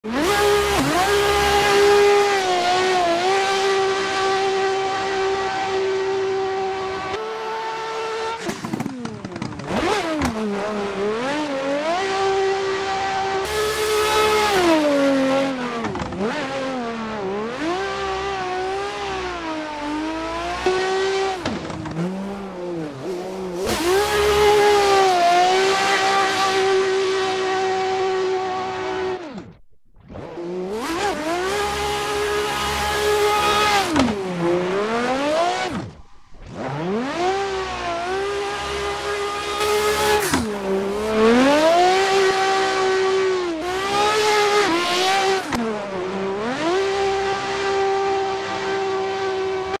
sliding about
not a good drifter at all, mostly just doing it for fun